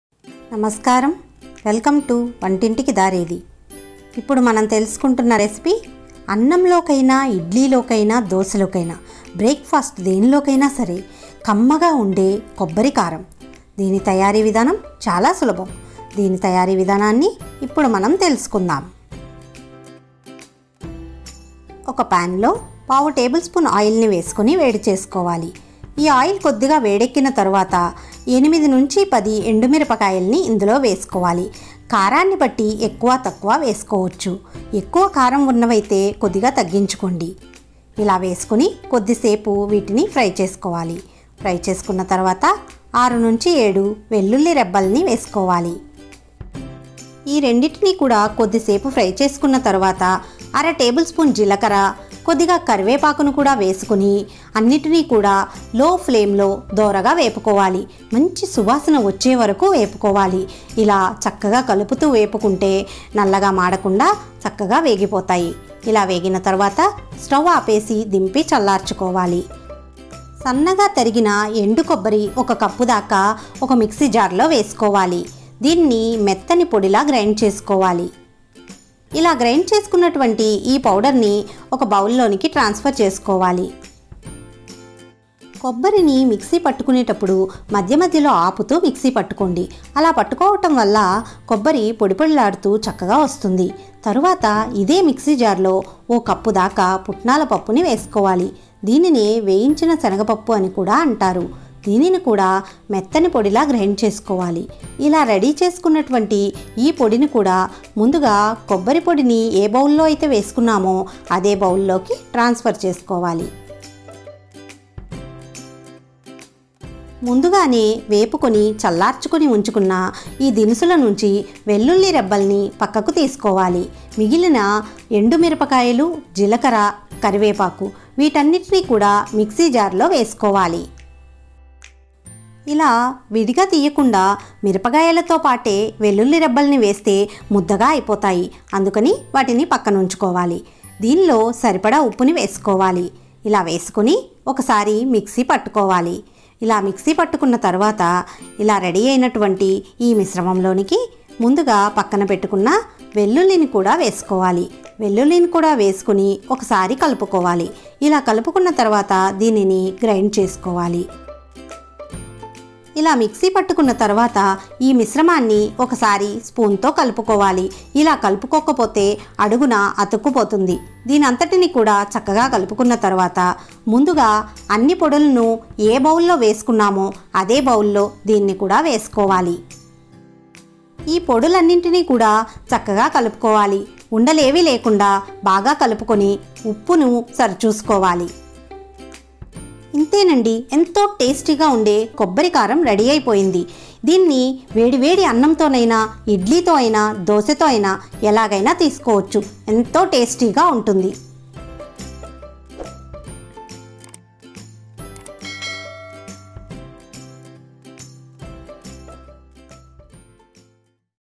వేడివేడి అన్నం లో నెయ్యి వేసి కొబ్బరి కారం కలుపుకుని తింటే జీవితంలో మర్చిపోరు మళ్లీమళ్లీ కావాలంటారు
వేడివేడి అన్నం లో నెయ్యి వేసి కొబ్బరి కారం కలుపుకుని తింటే జీవితంలో మర్చిపోరు మళ్లీమళ్లీ కావాలంటారు | Kobbari Karam

Kobbari Karam

Ingredients

Chopped Dry Coconut 1 cup
Dry Red chilli 8 to 10
Roasted Chana dal 1 cup
Garlic pods 6 to 7
Cumin seeds 1/2 tbsp
Curry leaves

Process

Pour 1/4 tbsp Oil in a pan and heat
Add dry red chilli, saute well, add garlic pods,mix well and add cumin seeds,  curry leaves and roast well on low flame
Turn off the stove and take out the bowl and allow it to cool down completely
Put 1 cup of chopped dry Coconut in a mixer jar and make a fine powder
Transfer the grinded powder into a bowl
Add roasted Chana dal and make a fine powder and add to the bowl
Add roasted red chilli, cumin seeds, curry leaves ,add enough salt and grind it
Into this add garlic, mix well and grind it again, mix the grinded powder and add it to the bowl
Mix all the powders well and adjust the salt and store it in an air tight container

Kobbari Karam Ready.

Copyright: All the contents of this channel is made by channel owner (Vantintiki Daredi). Do not use or copy any content without permission